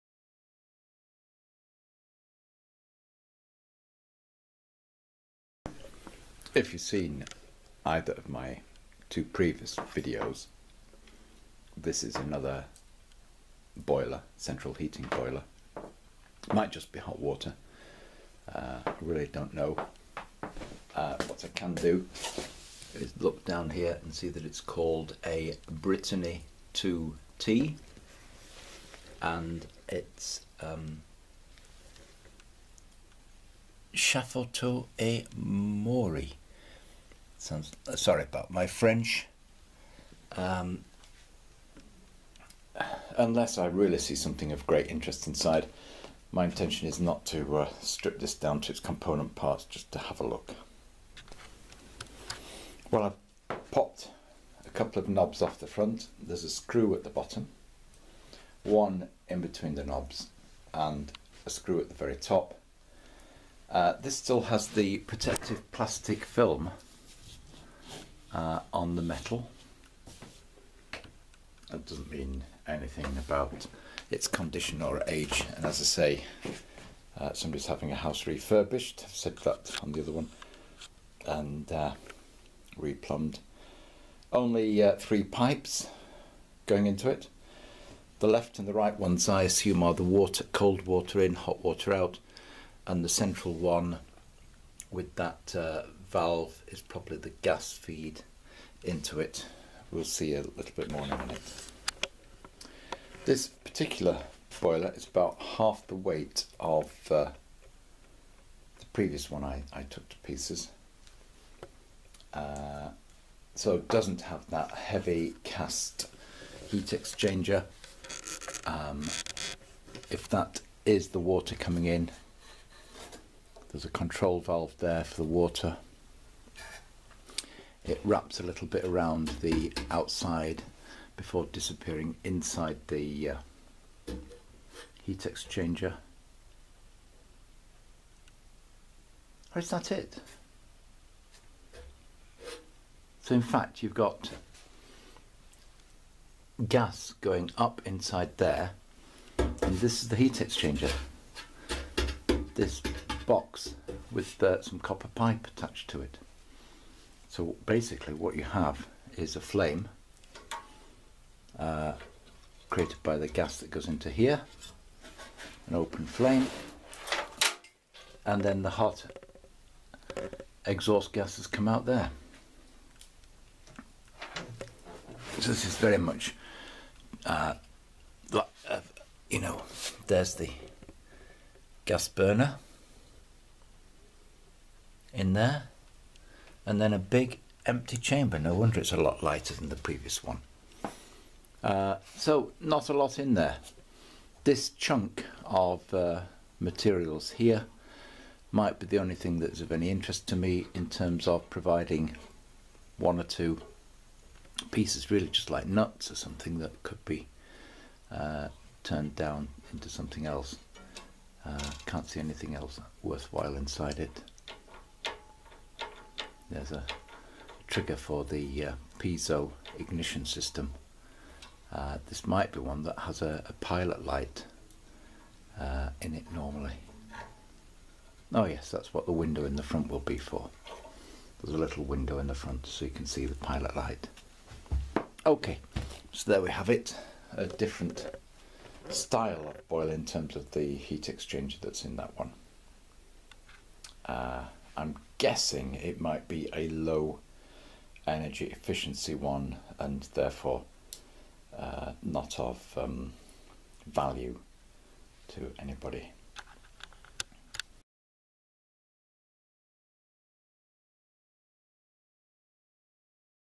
britony II T boiler strip down
I previously stripped down a Glow-worm open vent system boiler. This is a different type with a much lighter weight heat exchanger. It is made by Chaffoteaux et Maury. I think this may have been used to provide instant hot water for a bathroom extension.